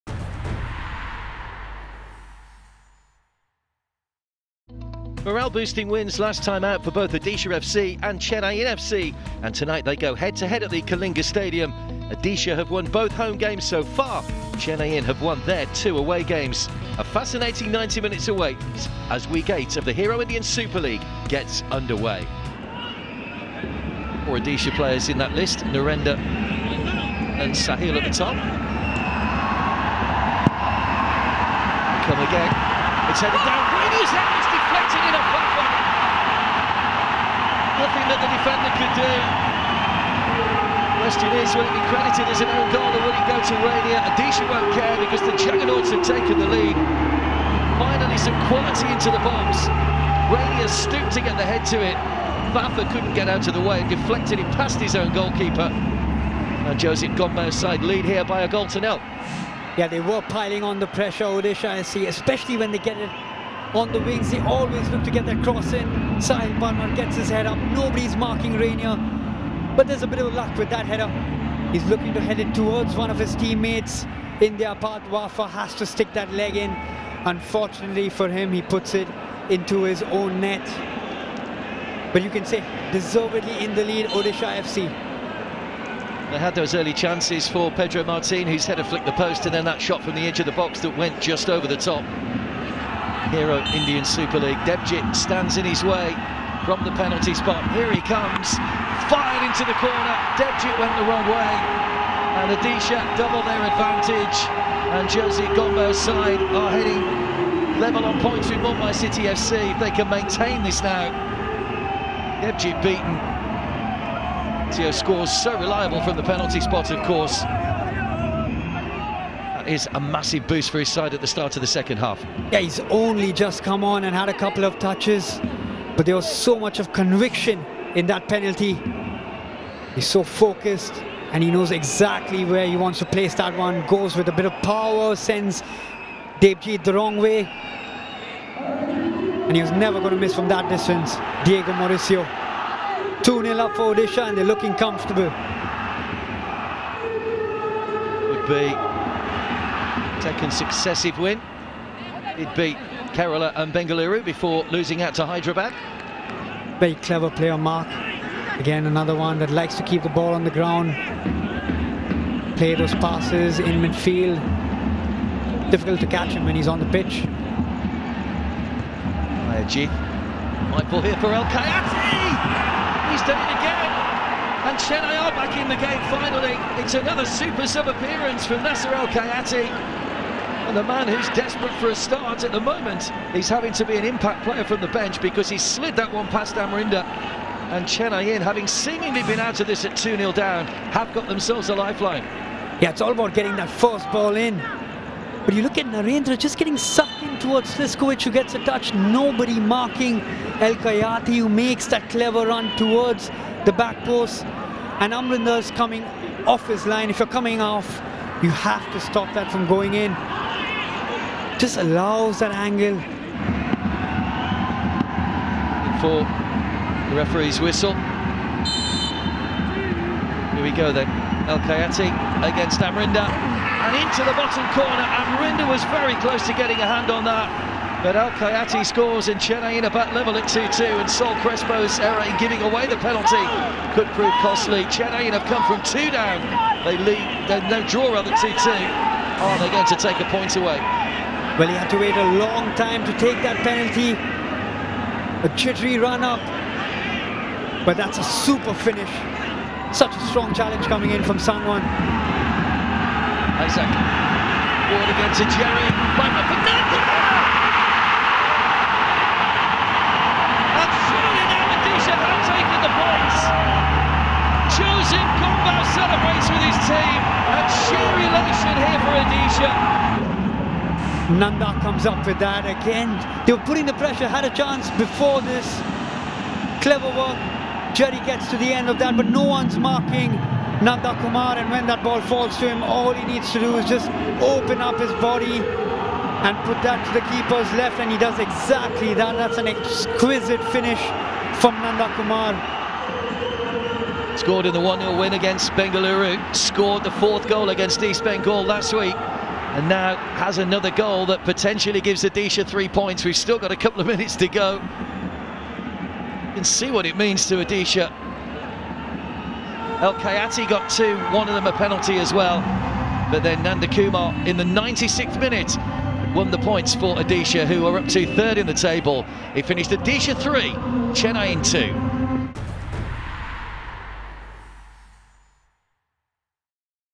ISL 2022-23 M36 Highlights: Odisha FC Vs Chennaiyin FC
Odisha FC and Chennaiyin FC ended in dramatic fashion in the Indian Super League (ISL) at the Kalinga Stadium in Bhubaneswar on Thursday. A 96th-minute Nandhakumar Sekar goal clinched a 3-2 win for the Juggernauts after Chennaiyin FC had scored the equaliser in the 94th minute via an Abdenasser El Khayati penalty.

Video Credit- FSDL